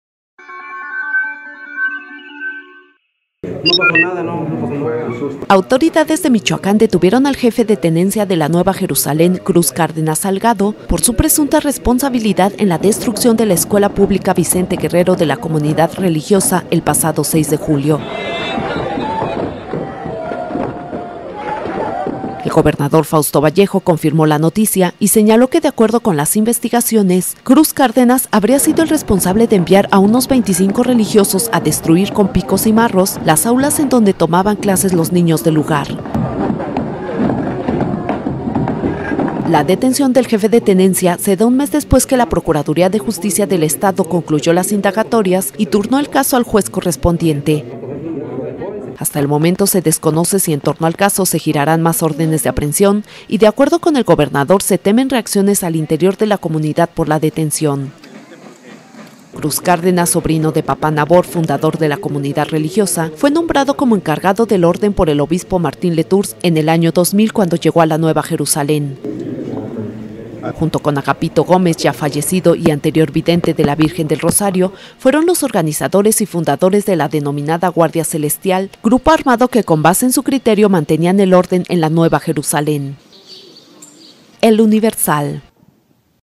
Detienen a responsable de destruir escuela en Nueva Jerusalén
De acuerdo con las investigaciones, Cruz Cárdenas habría sido el responsable de enviar a unos 25 religiosos a destruir con picos y marros las aulas en donde tomaban clases los niños.